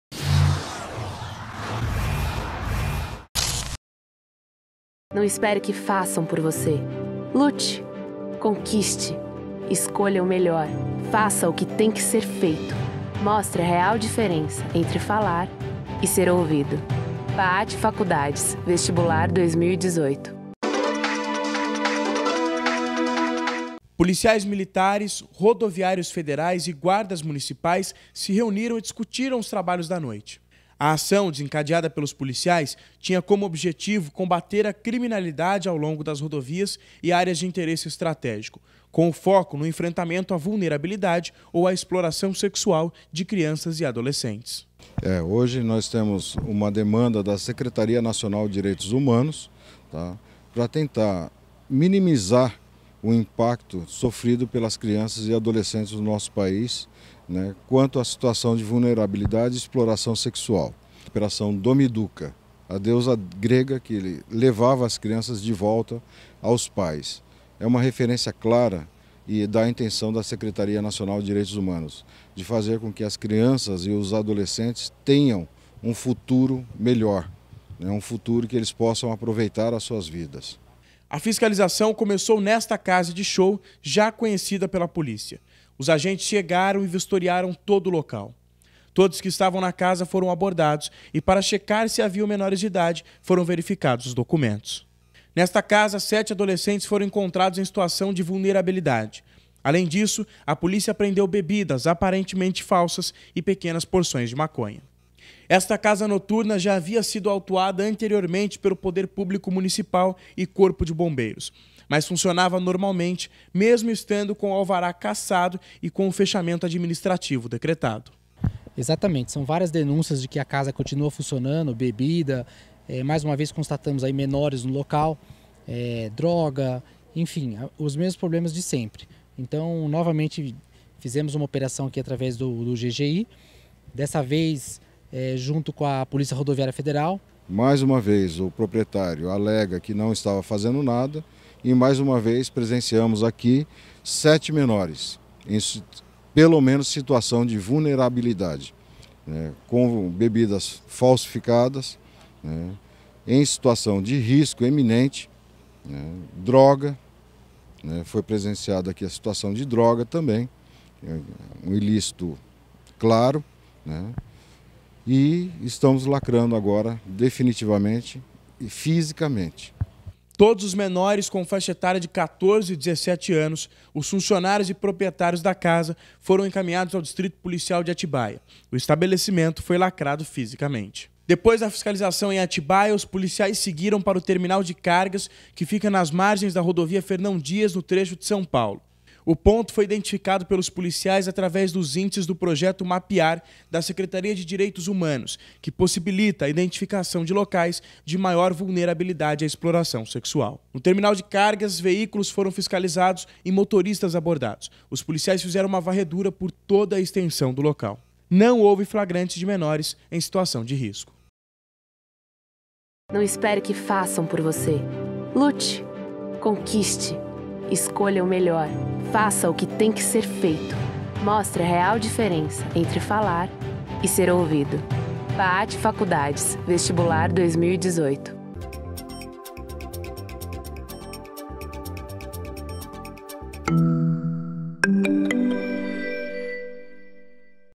GGI realiza operação contra exploração sexual
O Gabinete de Gestão Integrada de Segurança realizou na madrugada do último sábado a Operação "Domiduca". A ação tem como foco o enfrentamento à vulnerabilidade ou a exploração sexual de crianças e adolescentes. A fiscalização ocorreu em uma casa de show em Atibaia e em pontos identificados previamente pela polícia.